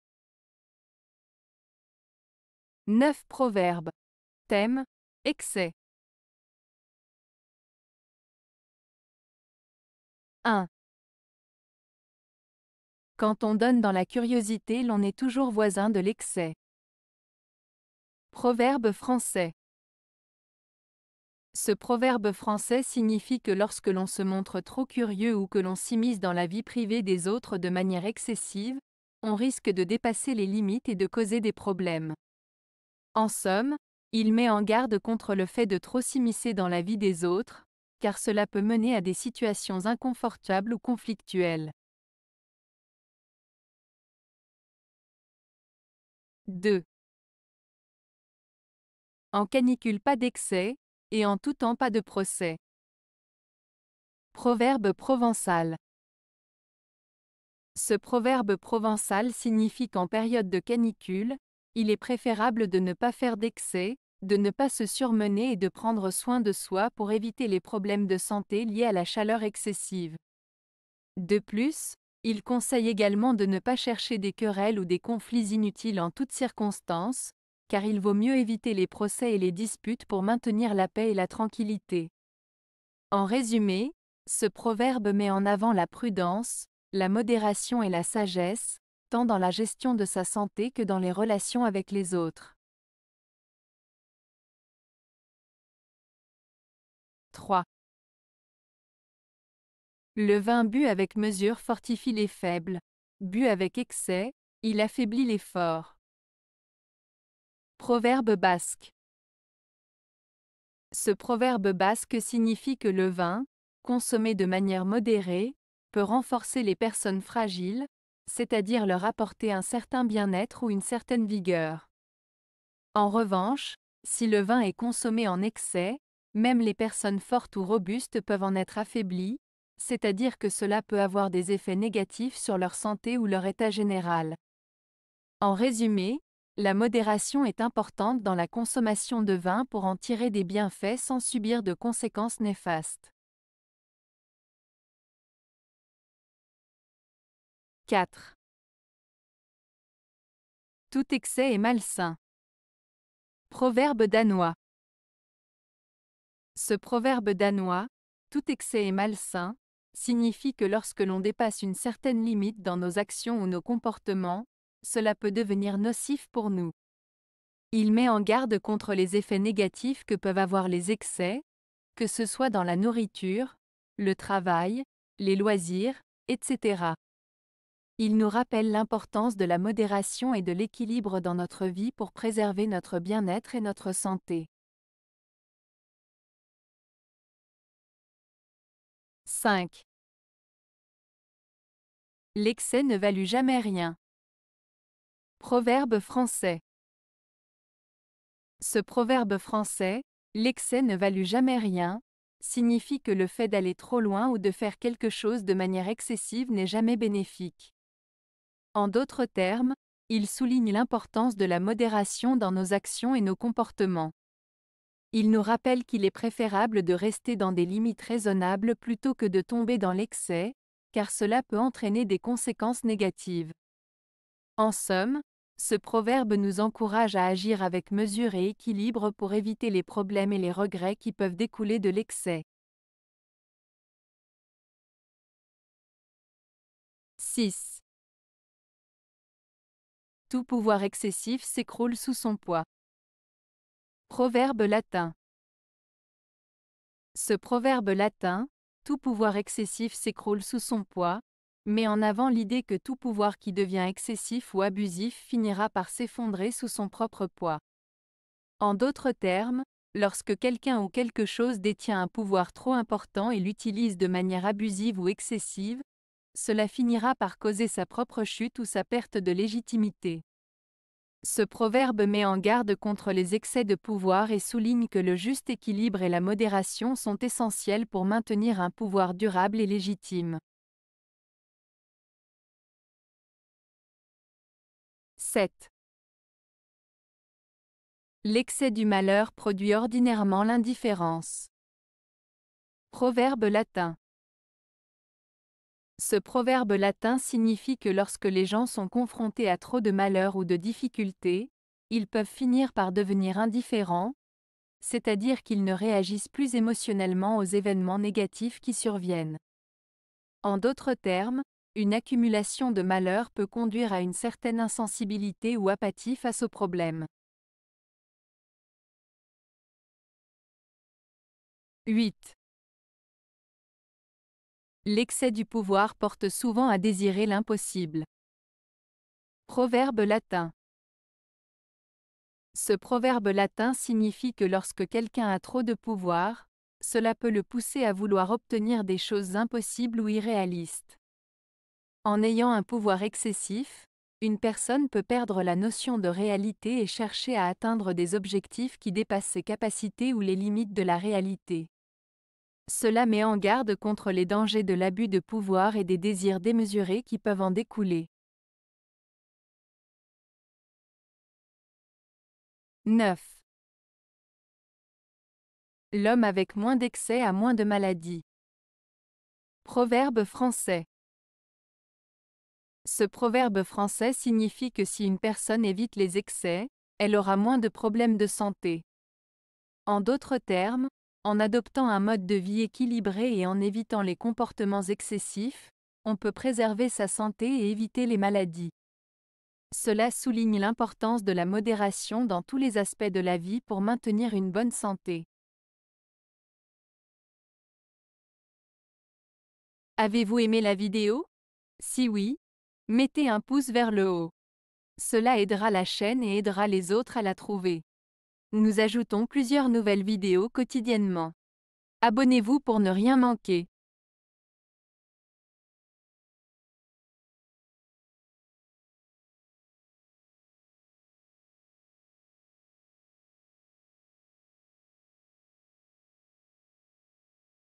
Proverbes - Excès (1 de 4)
00:00 Introduction
00:07 Proverbe Français - Quand on donne dans la curiosité l'on est toujours voisin de l'excès.
00:43 Proverbe Provençal - En canicule pas d'excès, et en tout temps pas de procès.
01:34 Proverbe Basque - Le vin bu avec mesure fortifie les faibles ; bu avec excès, il affaiblit les forts.
02:25 Proverbe Danois - Tout excès est malsain.
03:06 Proverbe Français - L'excès ne valut jamais rien.
03:54 Proverbe Latin - Tout pouvoir excessif s'écroule sous son poids.
04:42 Proverbe Latin - L'excès du malheur produit ordinairement l'indifférence.
05:19 Proverbe Latin - L'excès du pouvoir porte souvent à désirer l'impossible.
06:00 Proverbe Français - L'homme avec moins d'excès a moins de maladies.